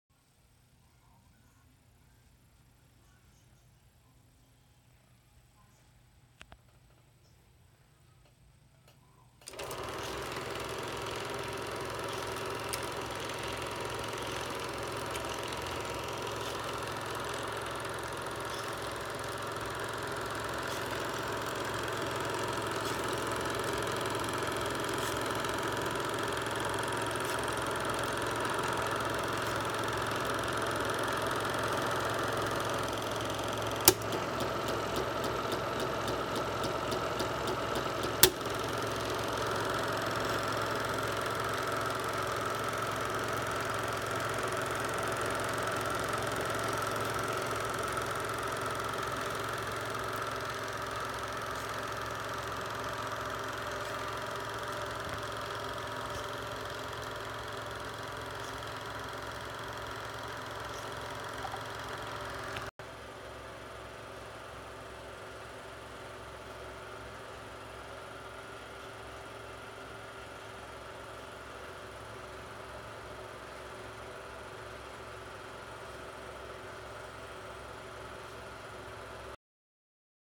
Canon Cine Projector Model T-1 Dual 8mm
The Canon T-1 was their top of the line dual 8mm movie projector. It was first released in April 1972. It was equipment with a Canon PS F 1.0 mm 17-28 mm zoom lens. The projector could also run slow motion, and had a variable speed selector range from 15-25 fps. Also standard was a halogen 150 watt bulb DNF type. Please note the footage at the end of this video was shot with a Pixel smartphone. It does not represent the quality of the actual projected image.